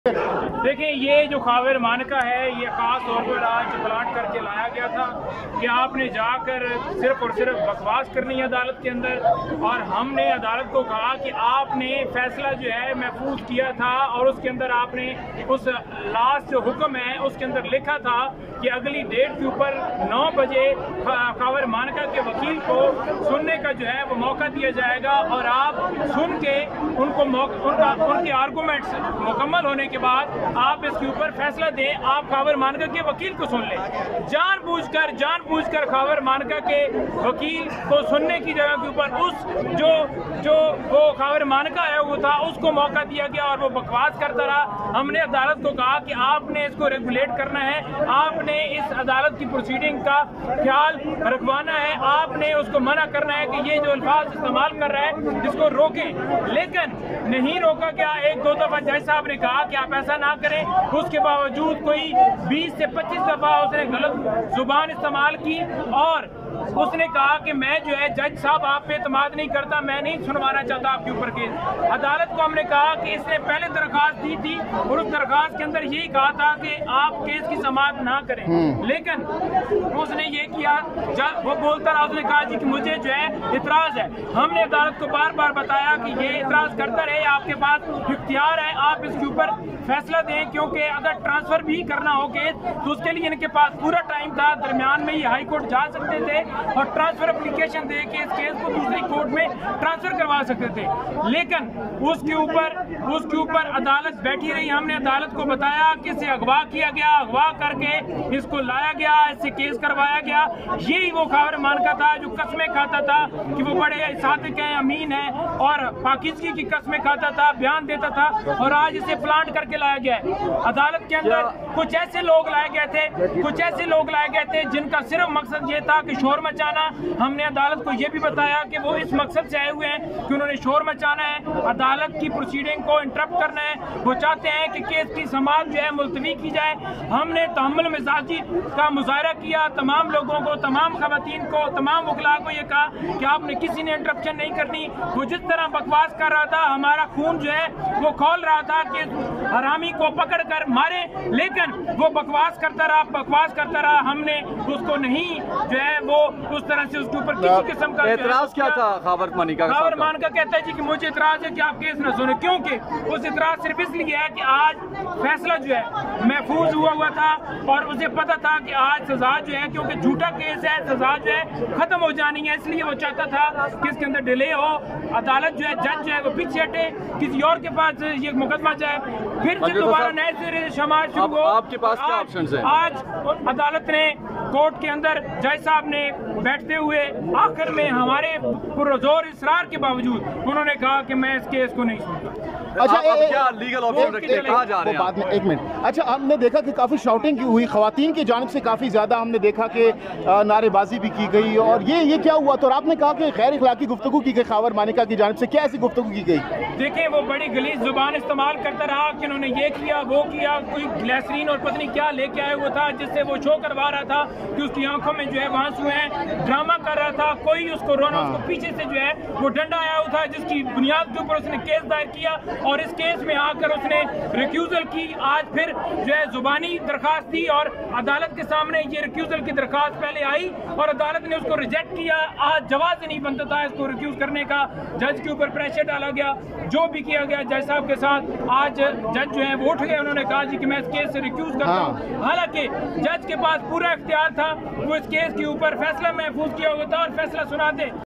Islamabad Court Big Decision | Imran Khan And Bushra Bibi Win | PTI Lawyer And Worker In Action
| News Updates